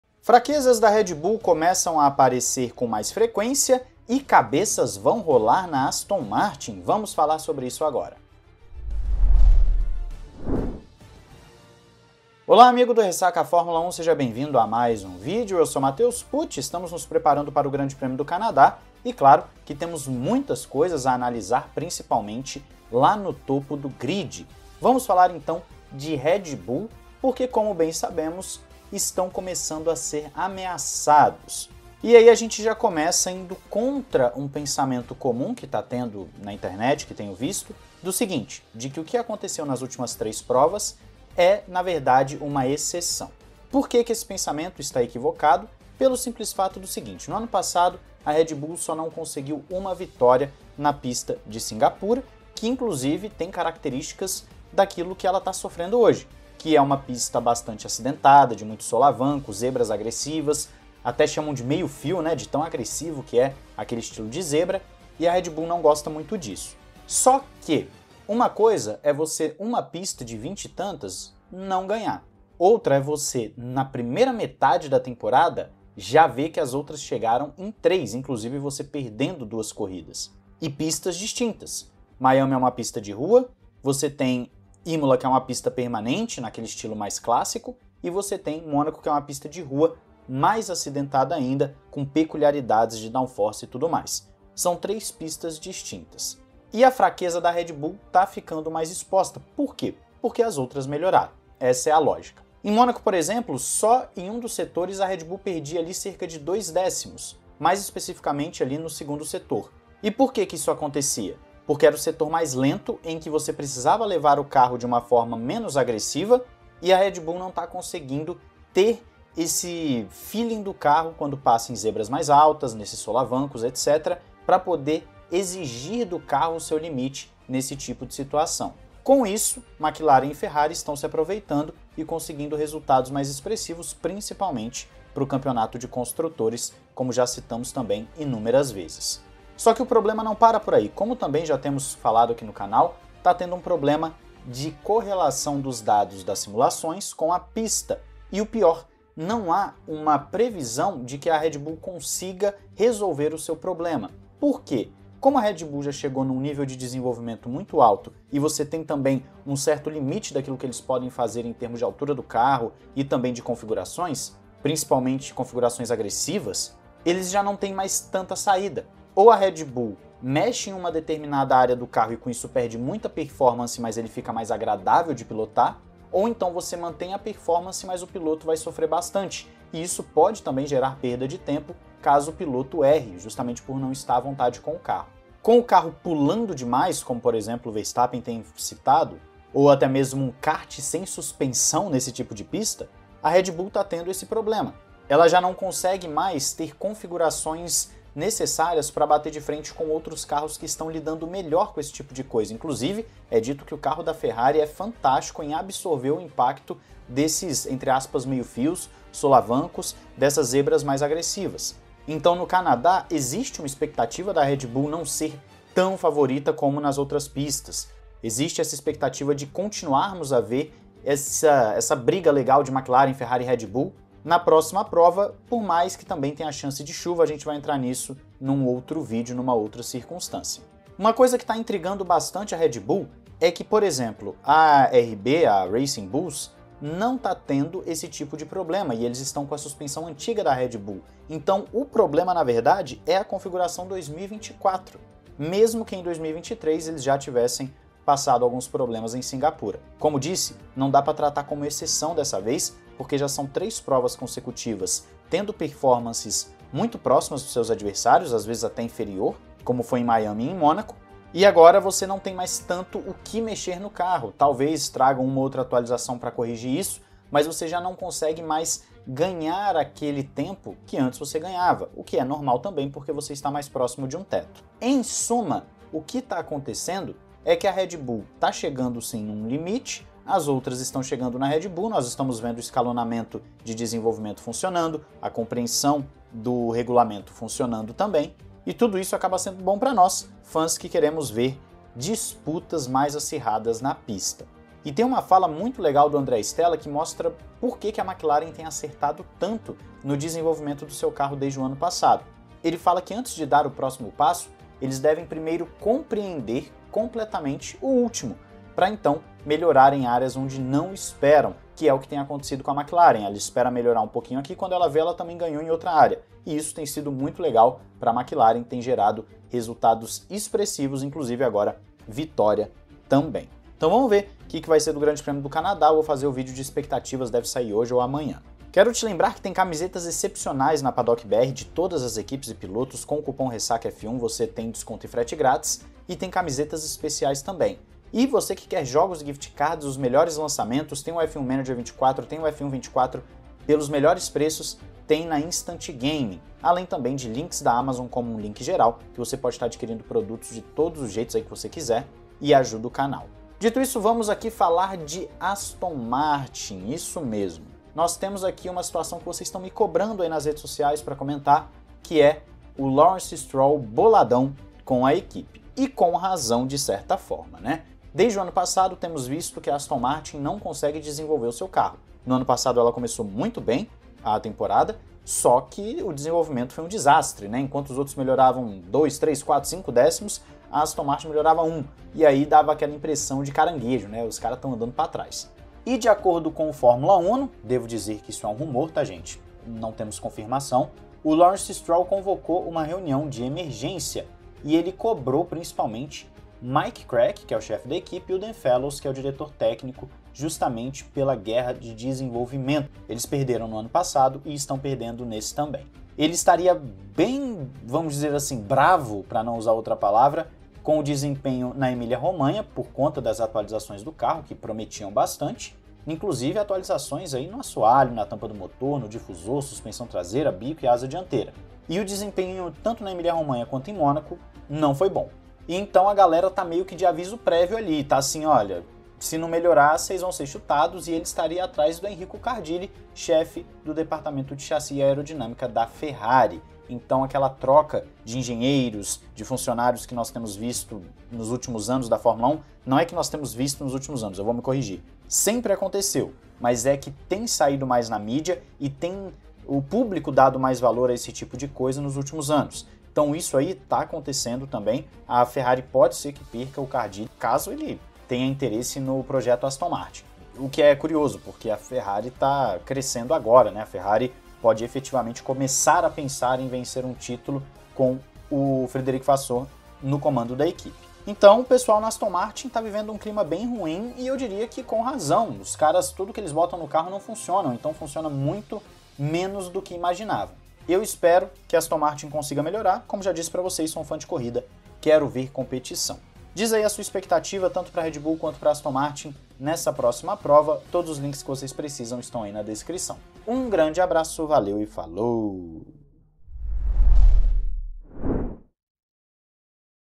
CABEÇAS VÃO ROLAR NA ASTON MARTIN, FINALMENTE / RED BULL COM FRAQUEZAS ACENTUADAS
Sergio Milani
@scuderiamilani (Twitter)

João 3:16

00:00 - Introdução
00:25 - Fraquezas da Red Bull mais acentuadas em 2024
02:25 - Não há previsão de melhora
03:52 - Suspensão é o detalhe
04:35 - Tudo funcionando
05:28 - Não perca as promoções!
05:53 - Lawrence Stroll bolado
07:05 - Cabeças vão rolar